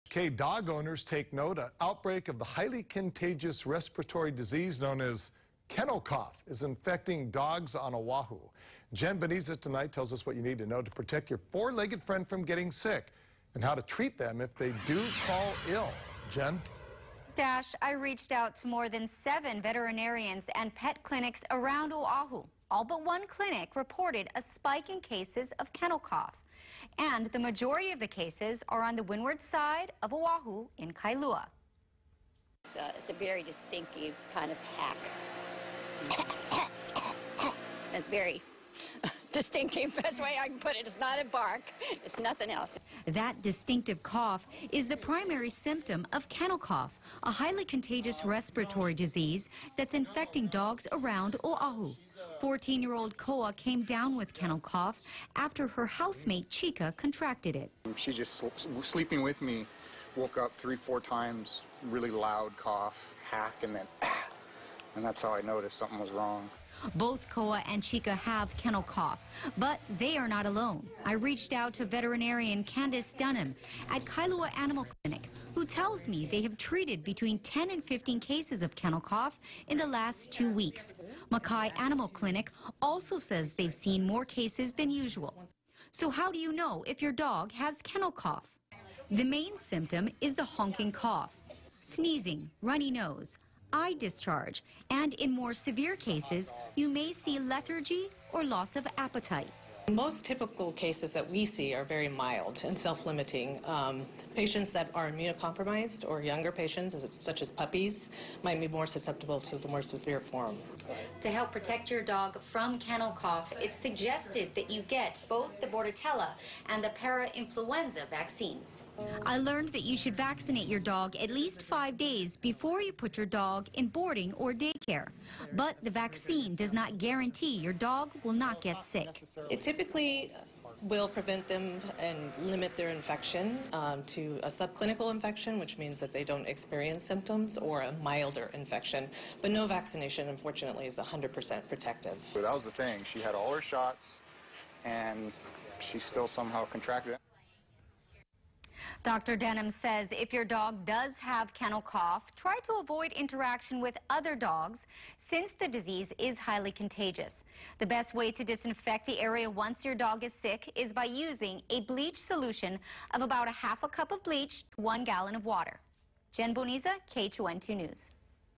Cases of highly contagious kennel cough spike in dogs on Oahu
The main symptom is the honking cough, but your dog may also sneeze, have a runny nose and eye discharge.